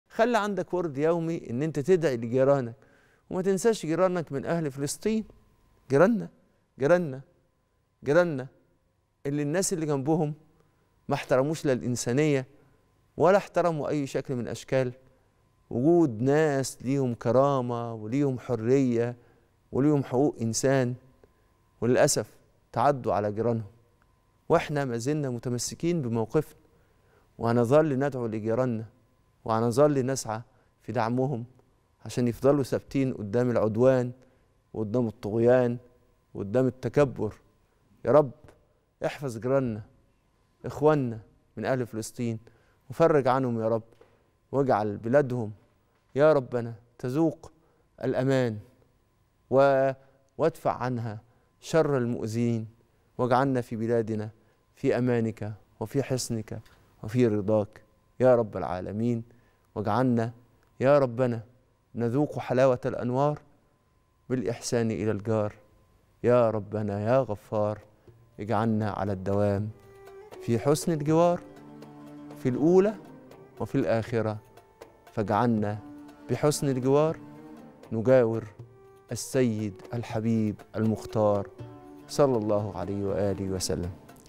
ماتنساش تدعي لجيرانك "أهل غـ ـزة" اللي الناس اللي جنبهم ماراعوش إنسانيتهم وآدميتهم
ولا تعسروا | ماتنساش تدعي لجيرانك "أهل غـ ـزة" اللي الناس اللي جنبهم ماراعوش إنسانيتهم وآدميتهم